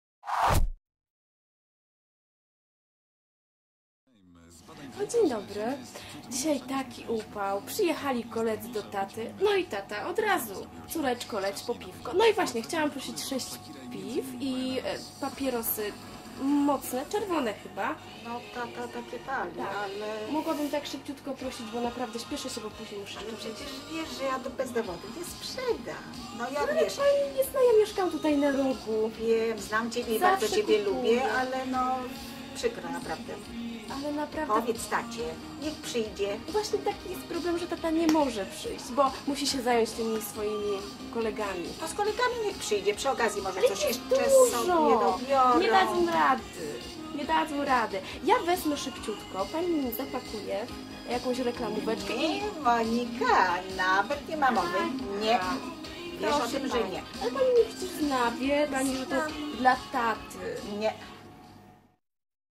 POZORY MYLĄ - sposoby nieletnich cz.2
Piotrków Trybunalski uczestniczy w ogólnopolskiej kampanii edukacyjnej organizowanej przez Ministerstwo Sprawiedliwości, Komendę Główną Policji i Związek Pracodawców Przemysłu Piwowarskiego. Celem akcji jest zwrócenie uwagi sprzedawców i świadków sprzedaży w sklepach na terenie całej Polski, że dorosły wygląd nie oznacza pełnoletniości upoważniającej do zakupu napojów alkoholowych. Działania prewencyjne kampanii mają na celu ograniczenie sprzedaży alkoholu osobom niepełnoletnim oraz przeciwdziałanie spożyciu alkoholu w tej grupie wiekowej. Filmy szkoleniowe mają pokazać w jaki sposób młodzież chce wymusić na sprzedawcy sprzedaż alkoholu oraz pokazać przyczyny zjawiska alkoholizmu wśród nieletnich.